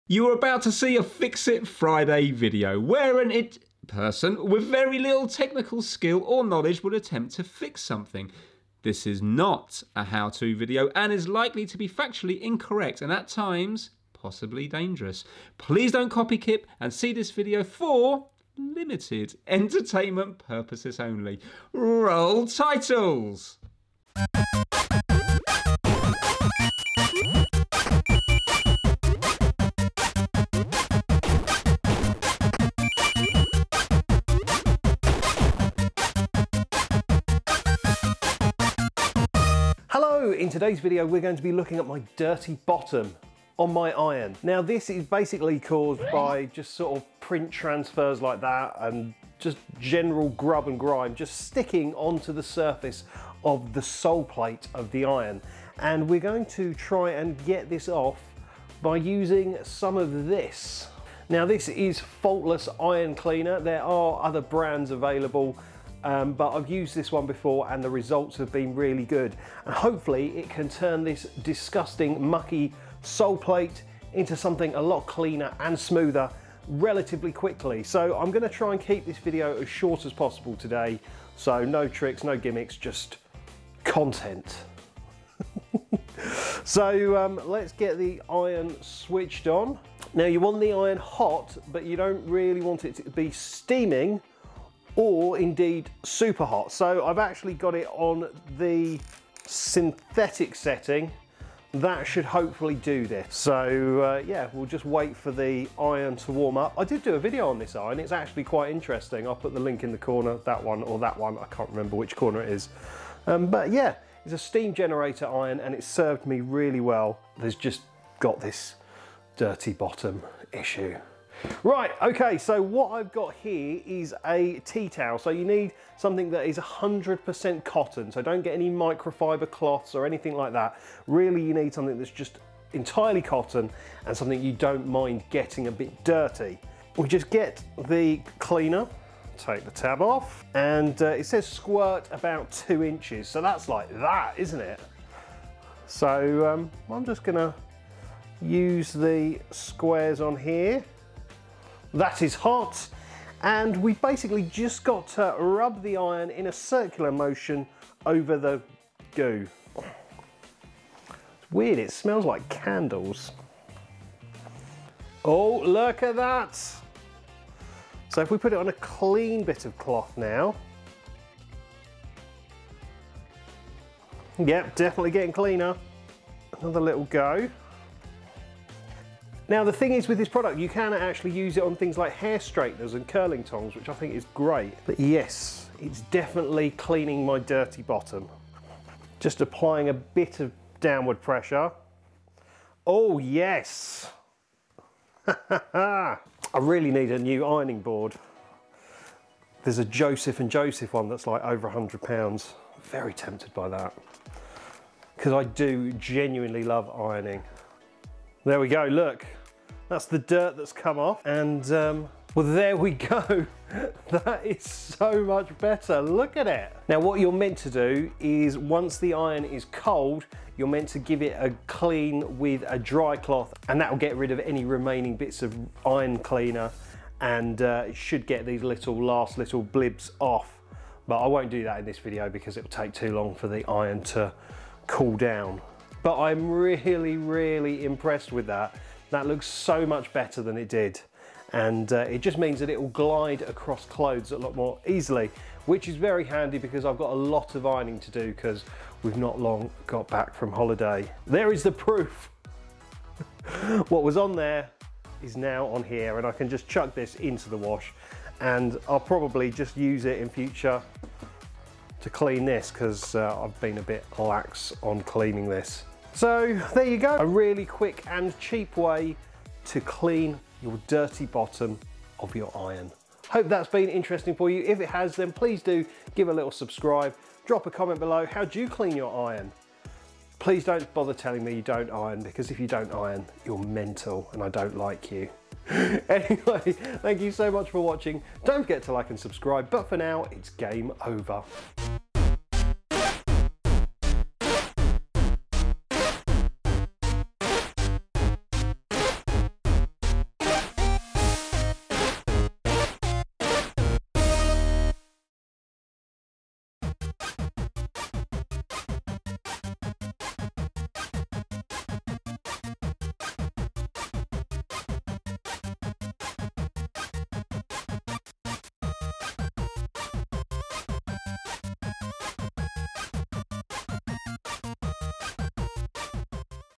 How to clean your Steam Iron - Making my Iron like new - Fix it Friday
In today's slightly random Fix it Friday, I'm trying to get the burnt in goo off the soleplate of my iron - this is the product I used (UK / USA

Can I clean my dirty bottom of the iron? Watch and see!

It's a super quick video, with a really simple fix!